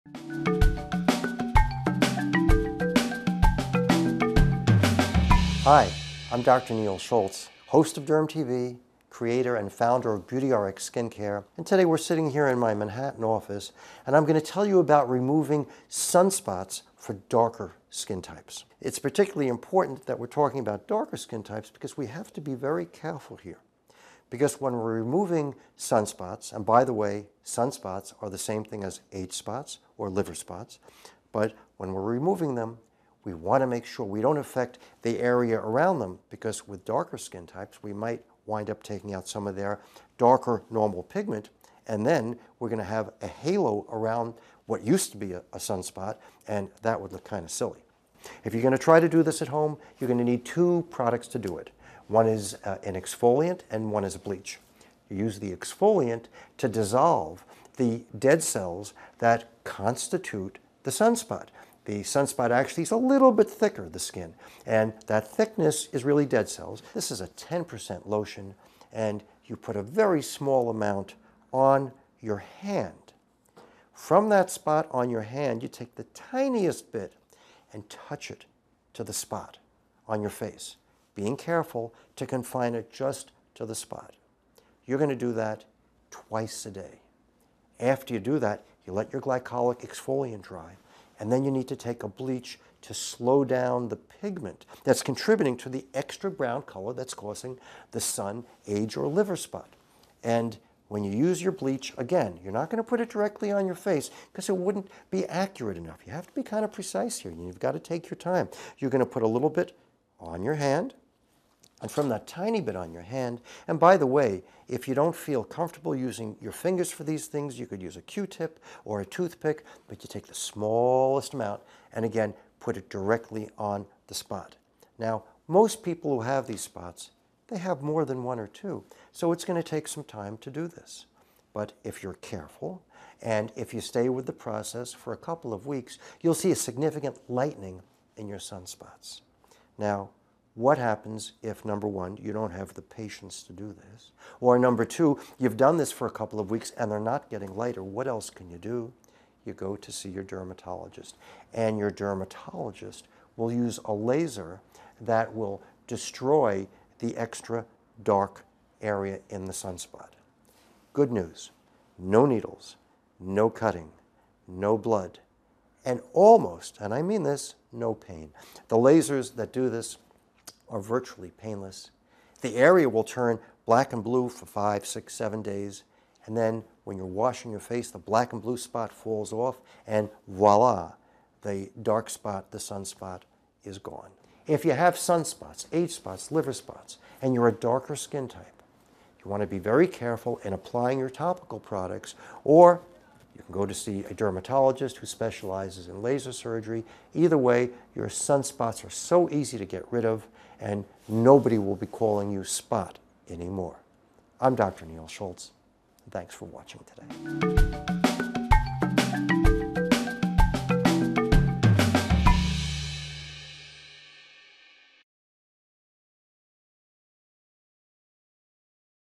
Removing Sun Spots for Darker Skin Types
Removing Sun Spots for Darker Skin Types. Part of the series: Skin Care & Dermatology. Removing sun spots for darker skin types requires you to think about them the same way you would liver spots. Remove sun spots for darker skin types with help from a leading, board-certified dermatologist in this free video clip.